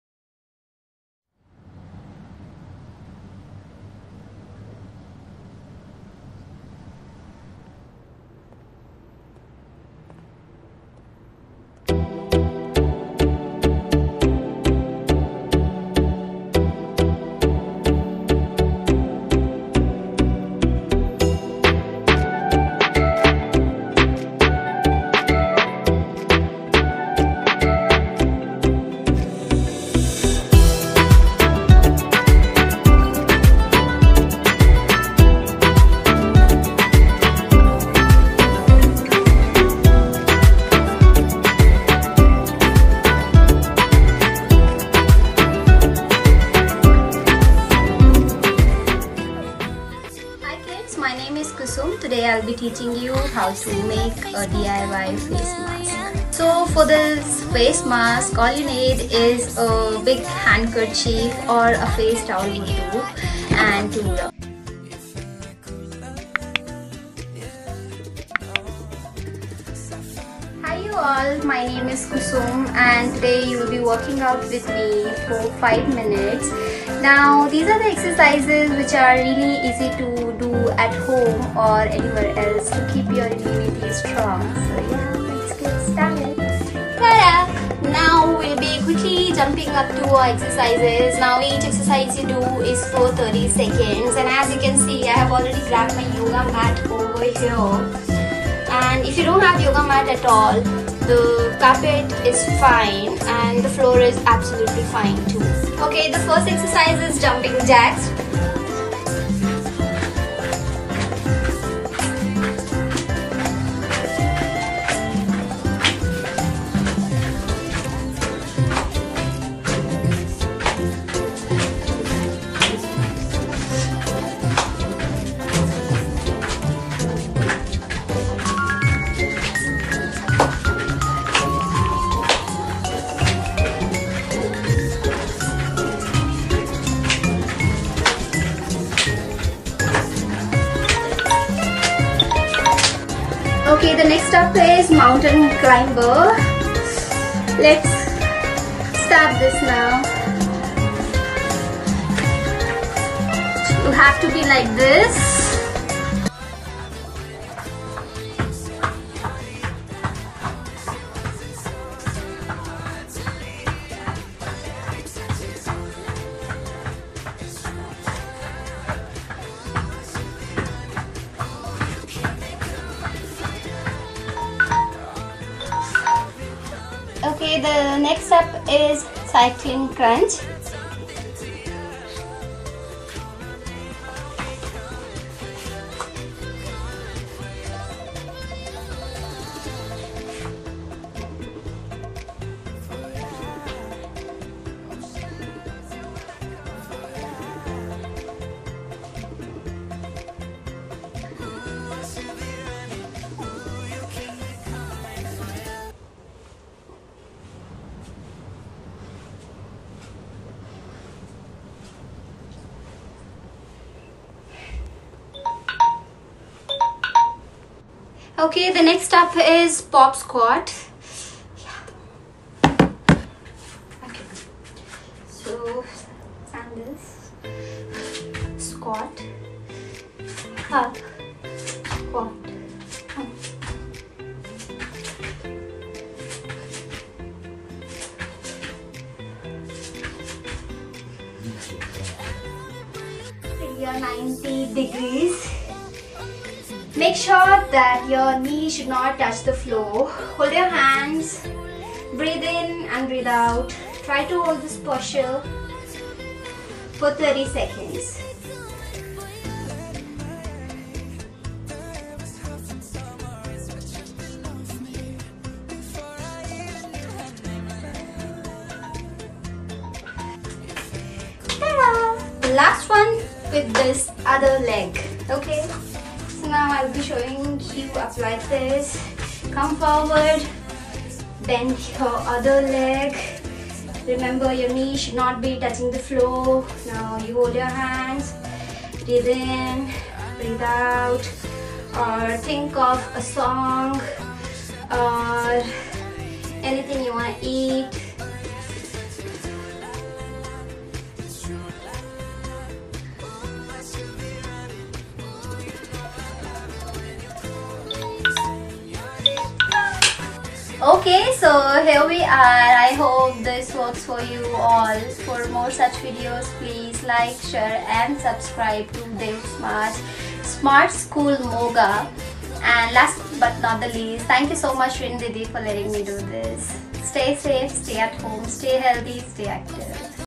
lockdown fitness fitness# stayfit
Kusum is an old student of our school working in Bangalore and a successful entrepreneur as well as a good example of woman empowerment. She is our family and helping us in homeschooling as she is master of extra curricular activities.wait for wonderful videoes from her side. She will show her all talents.